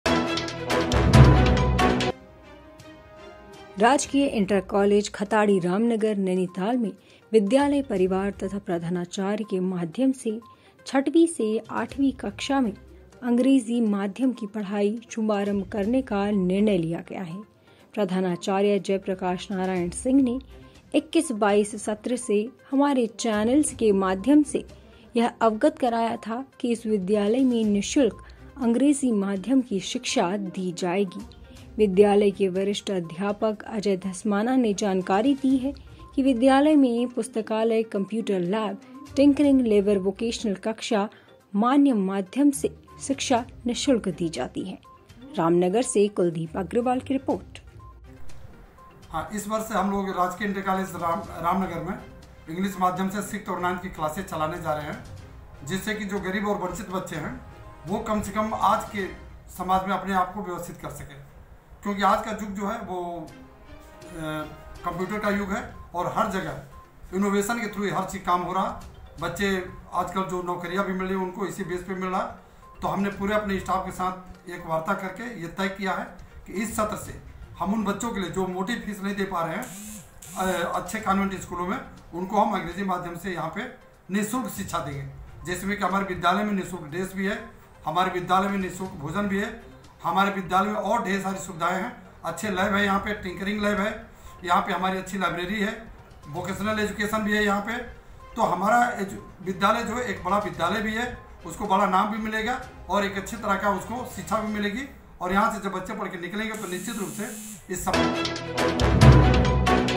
रामनगर के विद्यालय में माध्यम से 6व8वी अक्षा में अंग्रेजी माध्यम से पढ़ाई करने का निर्णय लिया है।
रामनगर।

कुलदीप अग्रवाल की रिपोर्ट।

एंकर। राजकीय इंटर कॉलेज खताड़ी रामनगर नैनीताल में विद्यालय परिवार तथा प्रधानाचार्य के माध्यम से 6व8 वी अक्षा में अंग्रेजी माध्यम से पढ़ाई शुभारंभ करने का निर्णय लिया है। प्रधानाचार्य जयप्रकाश नारायण सिंह ने 21 22 सत्र से  हमारे चैनल से माध्यम से अवगत कराया है कि इस विद्यालय में निशुल्क अंग्रेजी माध्यम से शिक्षा दी जायेगी। विद्यालय के वरिष्ठ अध्यापक अजय धस्माना ने जानकारी दी कि विद्यालय में पुस्तकालय कंप्यूटर लैब टिंकरिंग लेवर वोकेशनल कक्षा मान्य माध्यम से शिक्षा निशुल्क दी जाती है।